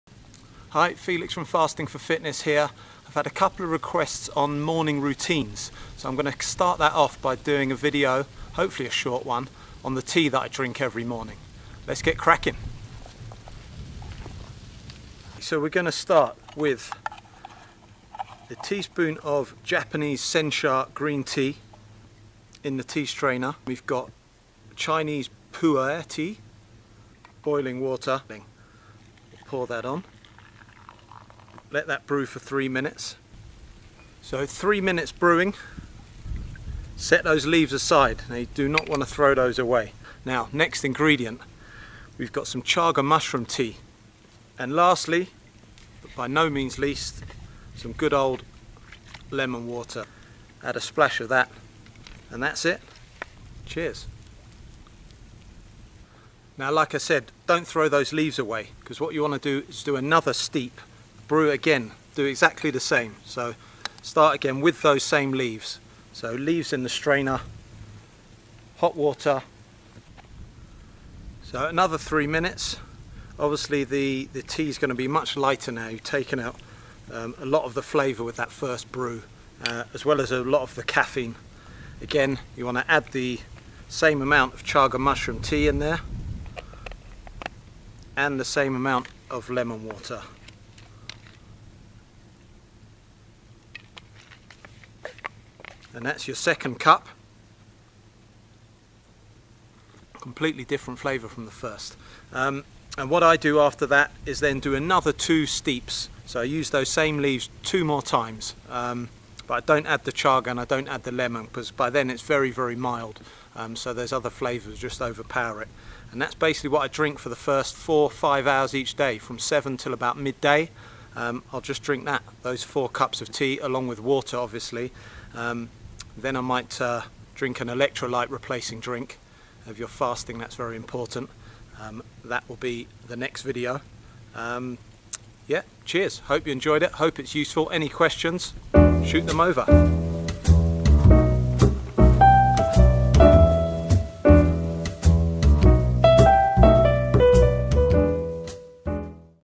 Intermittent Fasting Morning Tea Mix
This is a short video shot in the mountains of Serre Chevalier. Showing the preparation of my morning tea recipe. This mix is really good for Intermittent Fasting or just as a healthy start to your day...enjoy ☕️

These 2 teas - Pu-er Chinese Black Tea and Sencha Japanese Green Tea are really king of the teas in their respective countries.

Some of the health benefits of Pu-erh tea (pronounced ‘poo-air’)

* raise the level of good cholesterol (HDL) and lower the level of bad cholesterol (LDL.)
* speed up fat burning
* reduce stress
* The leaves go through a natural fermentation process before they're dried. This fermentation produces beneficial micro-organisms for your body
* Helps to cleanse the blood of toxins

Some of the health benefits of Sencha Tea:

* Powerful antioxidants
* Cardiovascular Benefits
* Cancer Prevention

Some of the health benefits of Chaga Mushroom - the list of health benefits of Chaga are pretty much endless but here are a few of the main ones...

* increased energy and endurance
* increased cardiovascular health
* increased intestinal and liver health
* promotes healthy blood sugar levels
* Protection from disease-causing Free-Radicals
* High in Antioxidants
* Stimulates the immune system
* Potent Anti-Viral
* and an Inflammation Reducer

Some of the health benefits of lemon water:

* Aids in digestion and detoxification
* Boosts energy
* Vitamin C